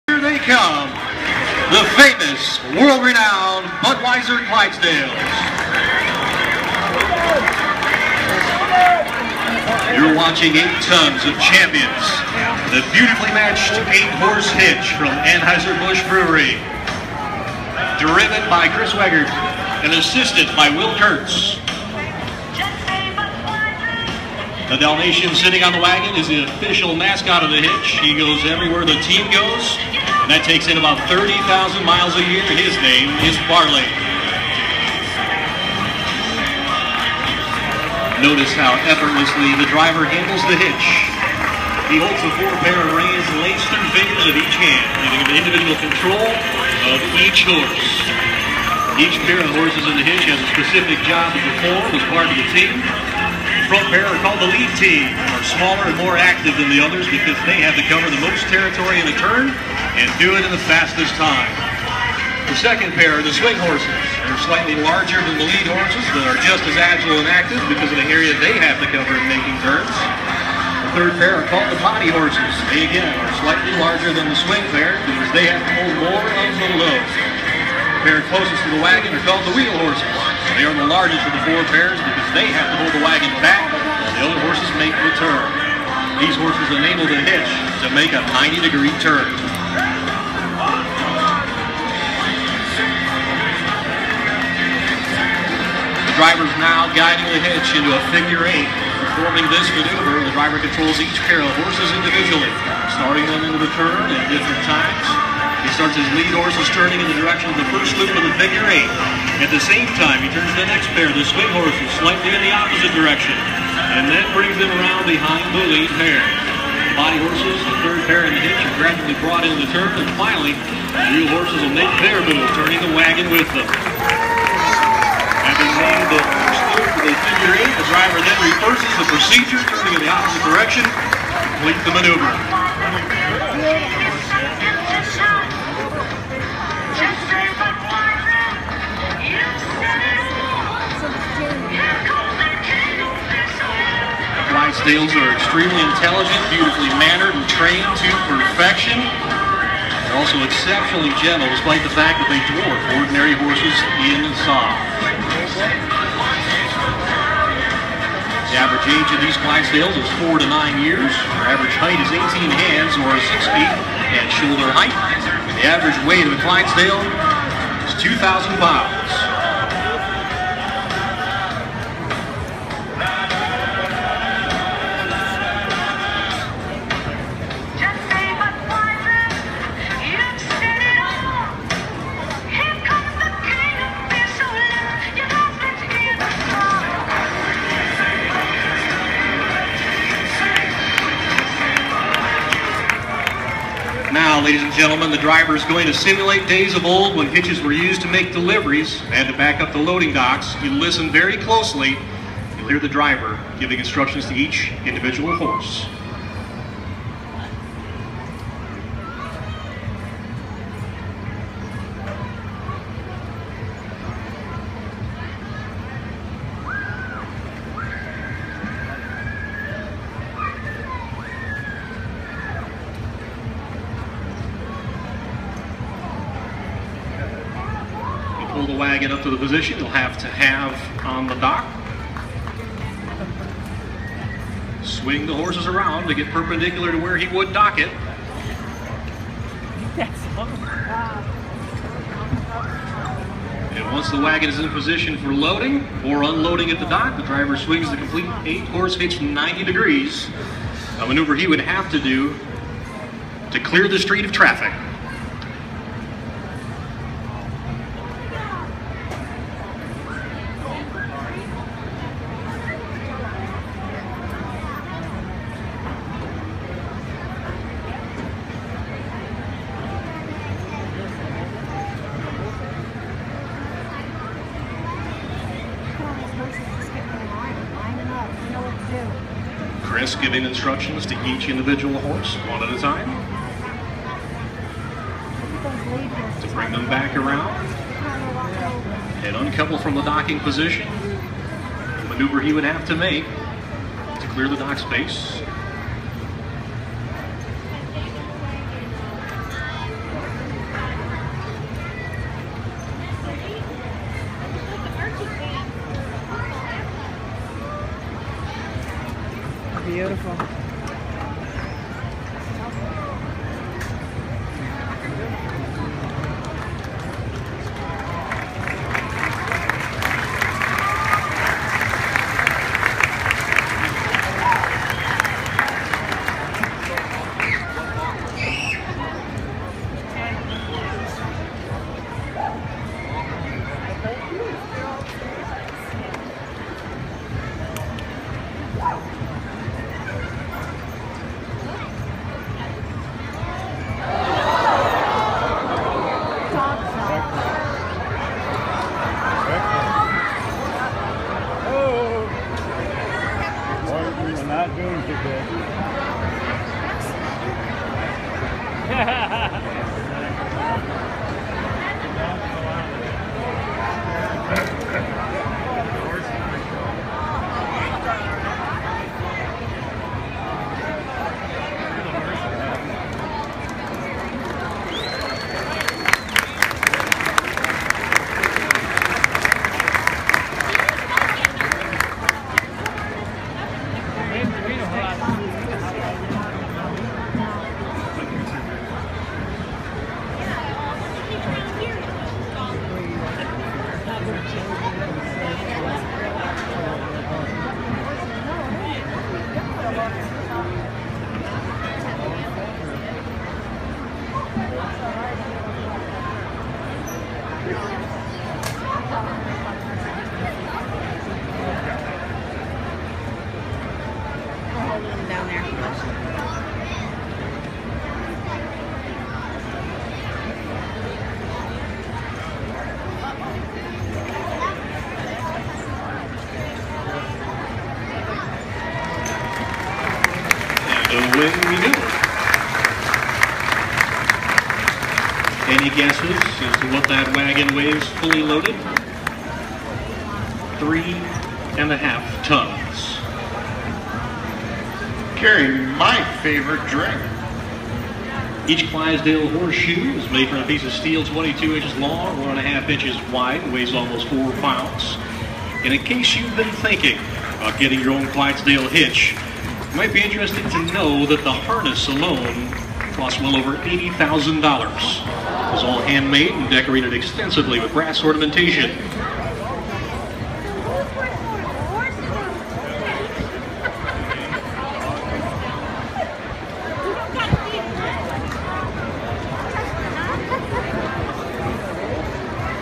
BUDWEISER CLYDESDALES, ARMADA FAIR, DOG FALLS OFF THE WAGON
budweiser clydesdales at armada fair, Dog falls off the wagon, but and super impressed the way the crew jumps to action, love these animals, got goose bumps when they passed up, the dog is fine, just scared